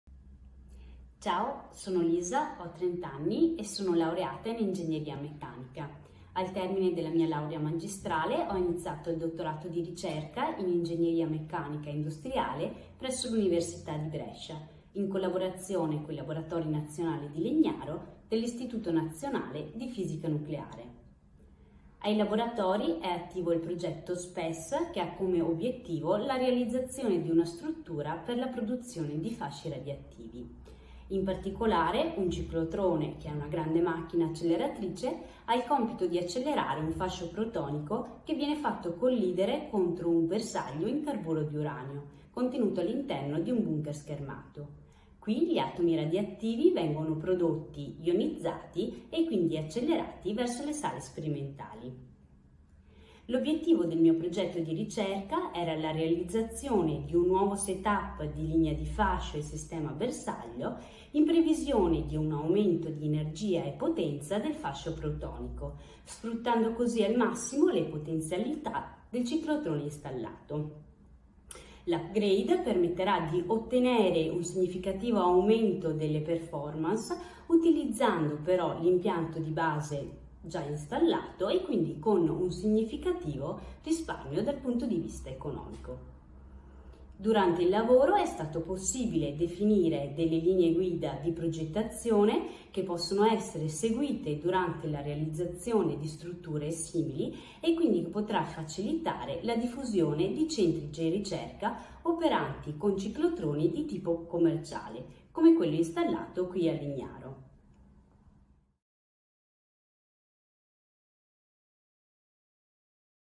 Lisa Centofante - PhD Student DRIMI Università degli Studi di Brescia
Titolo Tesi:
Progetto del canale di fascio e del sistema bersaglio per la nuova versione della macchina acceleratrice SPES ai Laboratori Nazionali di Legnaro